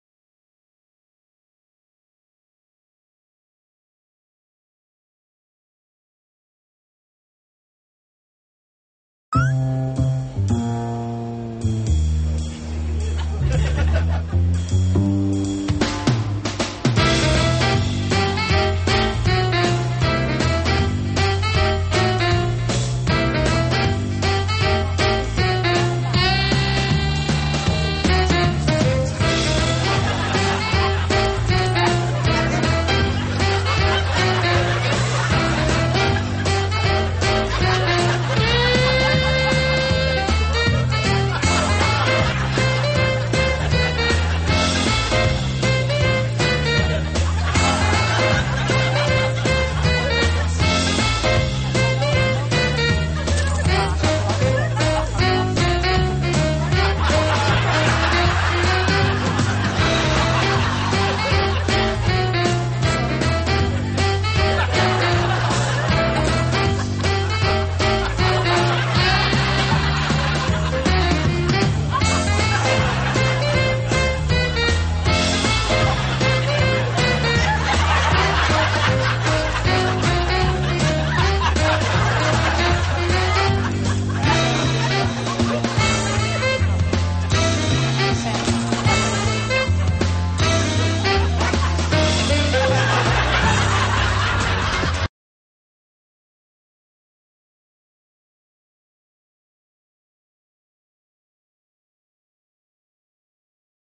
Miniskirt-Funny
look at tHose pervert guys' facial expressions. ha ha~~